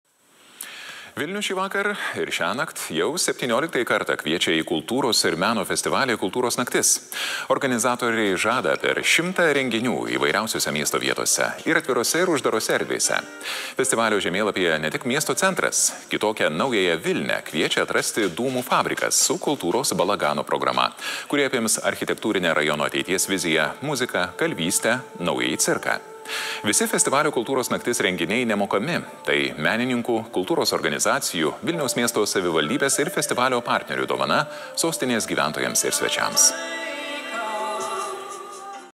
Kultūros naktis Dūmų gatvėje . LRT TV reportažas
2023 m. birželio 16 d.
Kultūros nakties renginys pavadintas „Kultūros balaganas Dūmų gatvėje“ vyko eisena nuo Vilniaus Naujosios Vilnios kultūros centro, kurią vedė mūsų mokyklos pučiamųjų orkestras "Domino" (vadovas mokytojas metodininkas Mindaugas Rarivanas).  Taip pat renginyje koncertavo akordeonų ansamblis bei solistai ( mokytoja ekpsertė Žana Gumenaja)